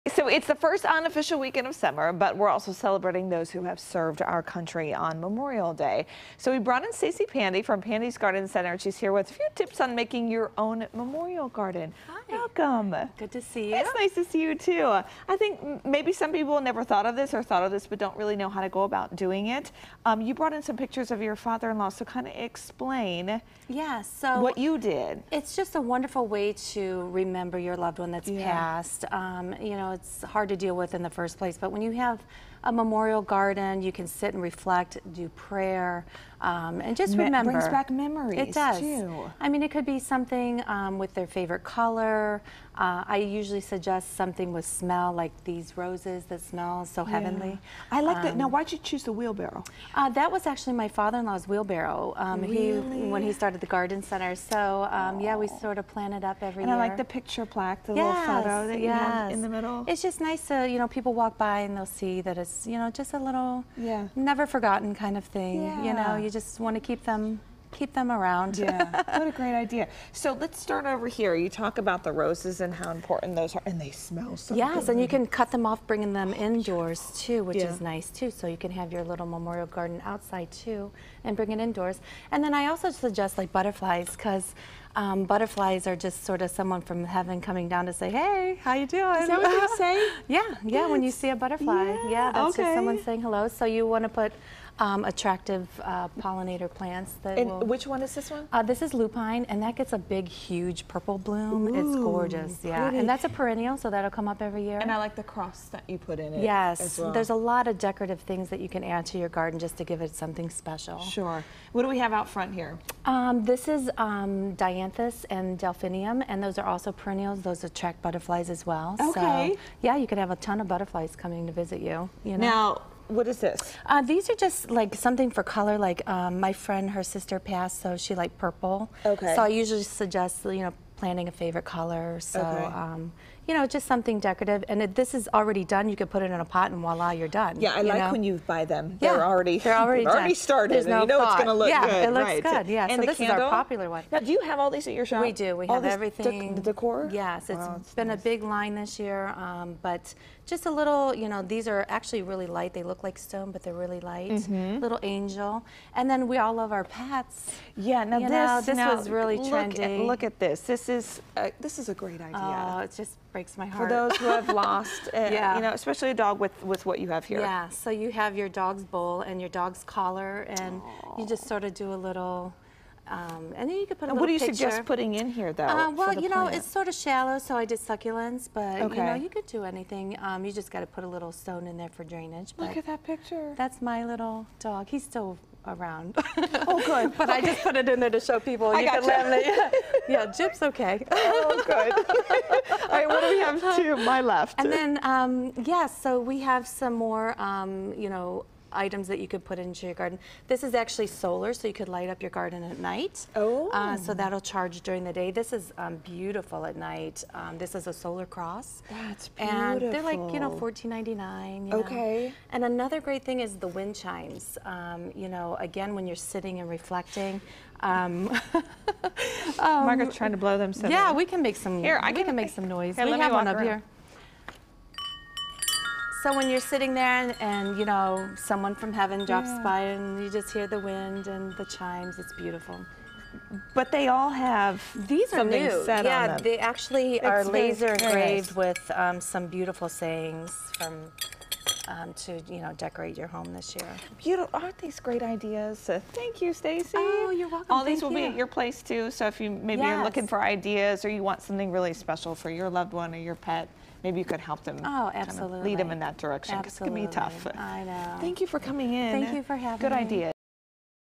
How to create a memorial garden in your backyard
A good way to honor a deceased loved one or a beloved pet is to create a peaceful and beautiful memorial garden in your backyard. It`s a place where you can go and reflect and remember. Stacy Pandy from Pandy`s Garden Center in Elyria showed Fox 8`s Kristi Capel some easy ways to get started.